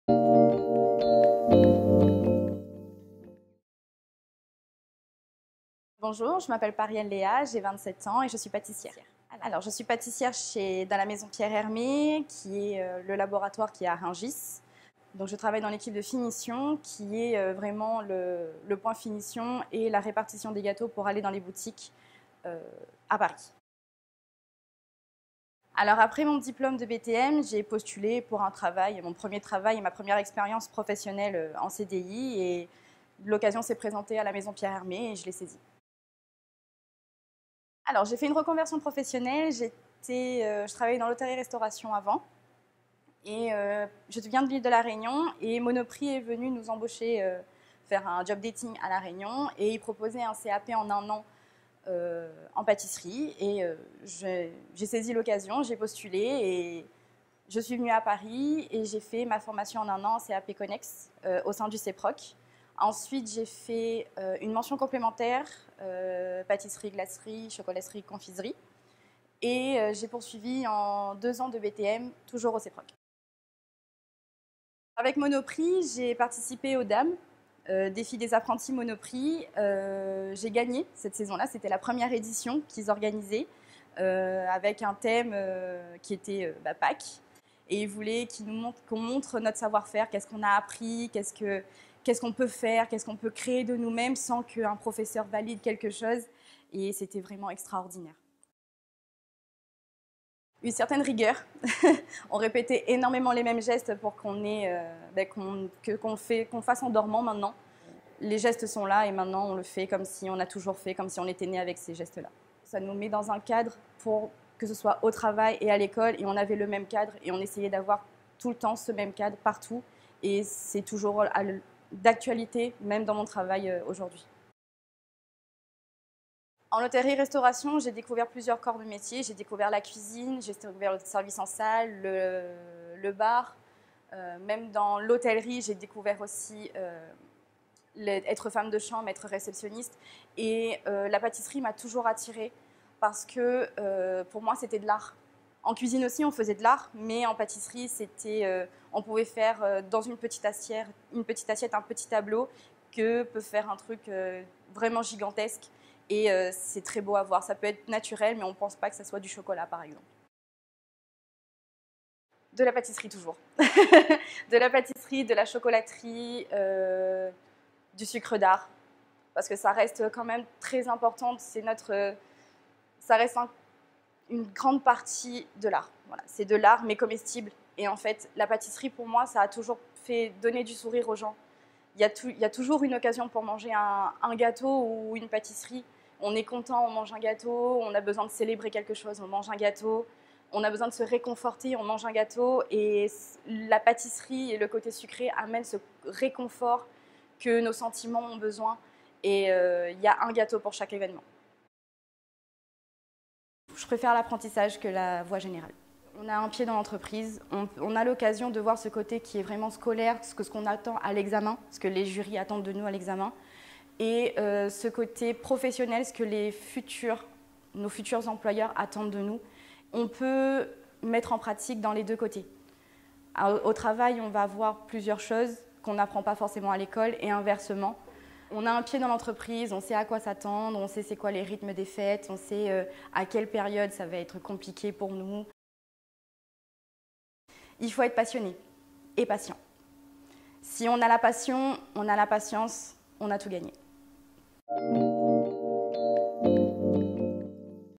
LEA PARIEL, LA VOIE DE LA PERSEVERANCE
A 27 ans, Léa Pariel entend aller au bout de sa passion pour la pâtisserie. Après un détour en zig-zag par l’hôtellerie-restauration sur l’île de Réunion, elle intègre le CAP pâtissier en 2018 et poursuit le cursus jusqu’au bout, décrochant un BTM pâtisserie quatre années plus tard. Un parcours sans faute qui lui ouvre, en 2022, les portes de la pâtisserie Pierre Hermé où elle occupe le pôle de la « finition » au sein du laboratoire de Rungis.  Dans cette interview, Léa Pariel évoque sa vision du métier et retrace son cursus au CEPROC, mettant l’accent sur la place déterminante que celui-ci a eu dans son début de carrière.